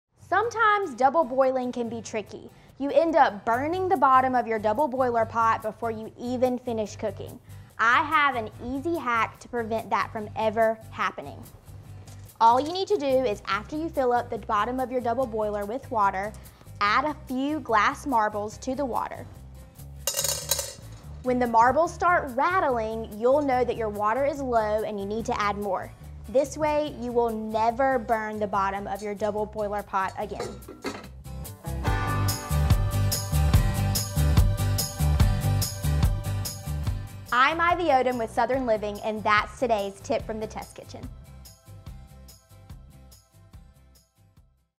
Learn How To Master Your Double Boiler | Tips From The Test Kitchen | Southern Living
Follow this easy double boiler trick for your best-ever chocolate covered strawberries.